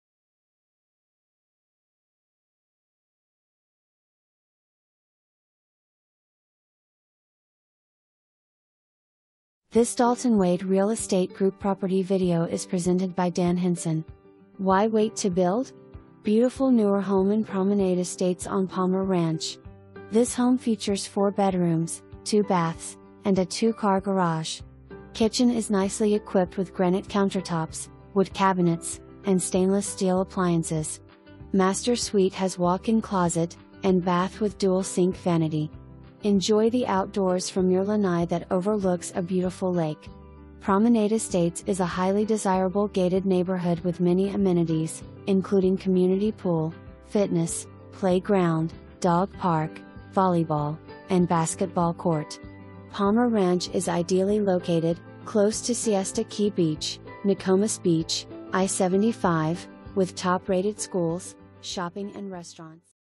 Dalton Wade Real Estate Group - 12521 NIGHT VIEW DRIVE
Why wait to build? Beautiful Newer Home in Promenade Estates On Palmer Ranch! This home Features 4 bedrooms, 2 baths, and a 2 car garage. Kitchen is nicely equipped with granite countertops, wood cabinets, and stainless steel appliances. Master Suite has walk in closet, and bath with dual sink vanity. Enjoy the outdoors from your lanai that overlooks a beautiful lake. Promenade Estates is a highly desirable gated neighborhood with many amenities, including Community Pool, Fitness, Play Ground, Dog Park, Volleyball, and Basketball court. Palmer Ranch is ideally located, close to Siesta Key Beach, Nokomis Beach, I-75, with top rated schools, shopping and restaurants.

2 full bath

Dan Henson
Dalton Wade Real Estate Group